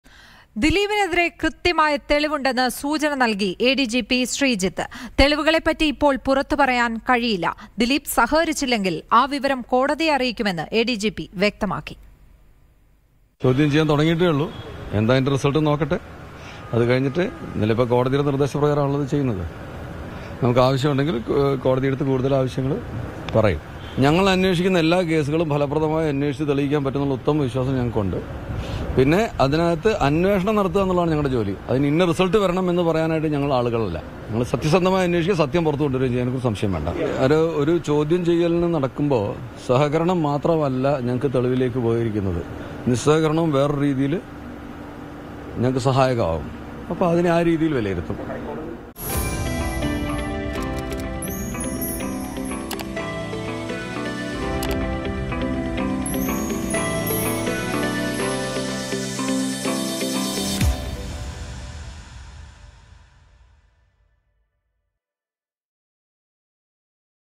'ദിലീപിനെതിരെ കൃത്യമായ തെളിവുണ്ട്, ഇപ്പോൾ വെളിപ്പെടുത്താനാകില്ല' | S Sreejith
'ദിലീപിനെതിരെ കൃത്യമായ തെളിവുണ്ട്, ഇപ്പോൾ വെളിപ്പെടുത്താനാകില്ല. സഹകരിച്ചില്ലെങ്കിൽ കോടതിയെ അറിയിക്കും'; അന്വേഷിക്കുന്ന കേസ് വിജയിക്കുമെന്ന് ഉത്തമവിശ്വാസമുണ്ടെന്ന് എസ്.ശ്രീജിത്ത്.
Actress Attack Case: Crime branch ADGP S Sreejith on Dileep interrogation